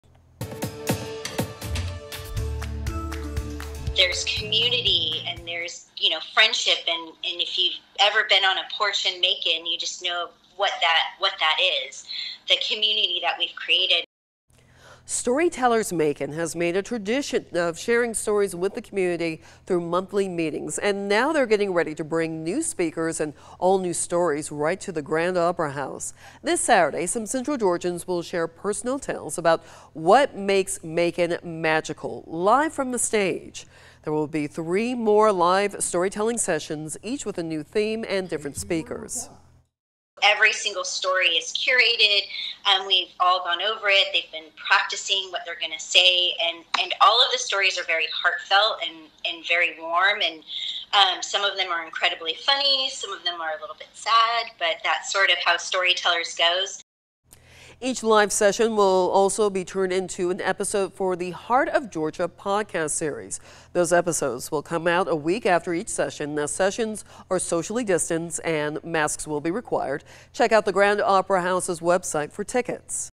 Storytellers Macon brings live sessions and podcast to the Grand Opera House
13WMAZ News is the #1 station for news and weather in the Central Georgia area, serving people in Dublin, Macon, Milledgeville and Warner Robins.